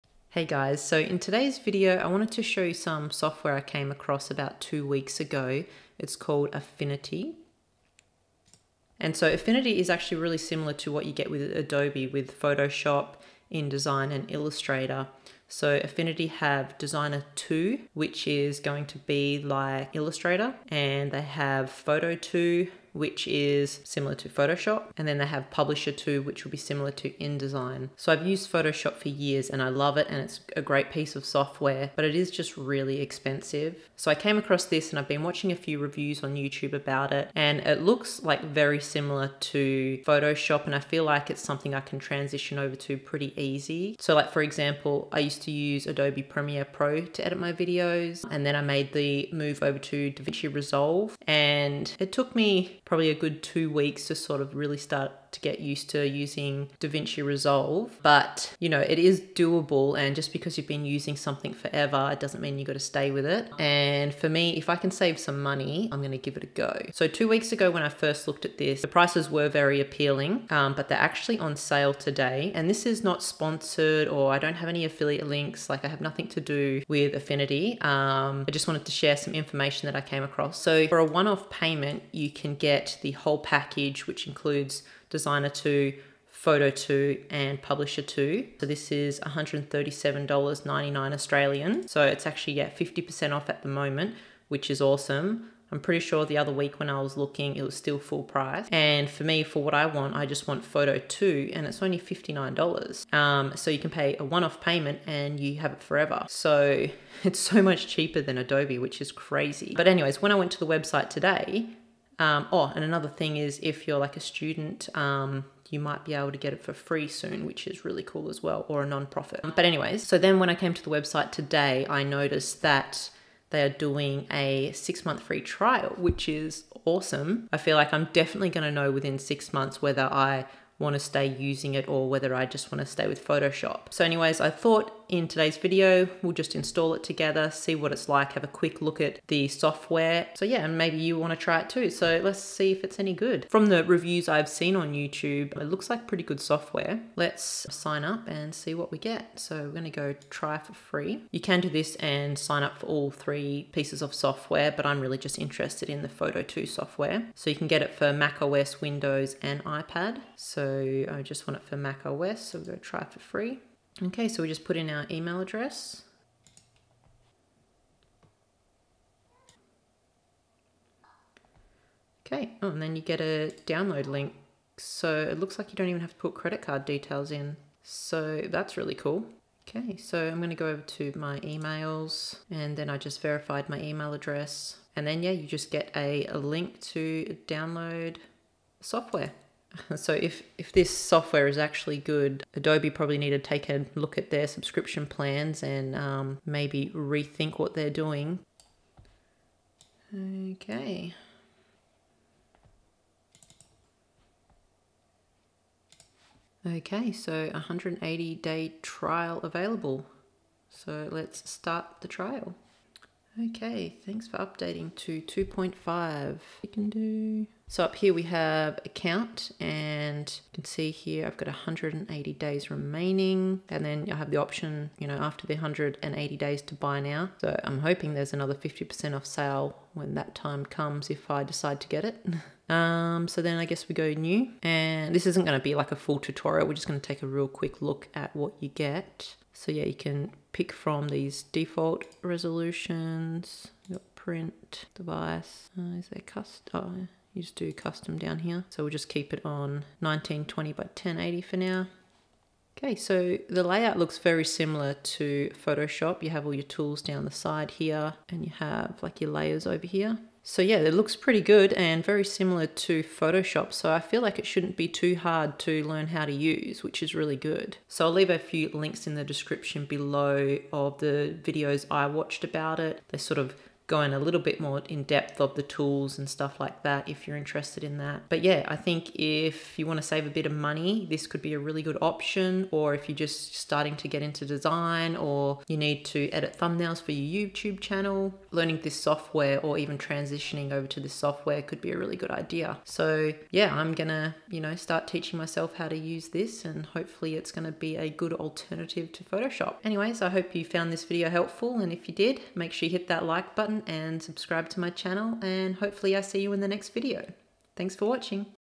Can Affinity Photo 2 Replace Photoshop? Let's Sign Up for the 6-Month Free Trial
In this video, I explore whether Affinity Photo 2 can replace Adobe Photoshop. We'll discuss the benefits of a one-time purchase and take advantage of the 6-month free trial. Join me as I install Affinity Photo 2 and consider if it's a viable alternative to Photoshop.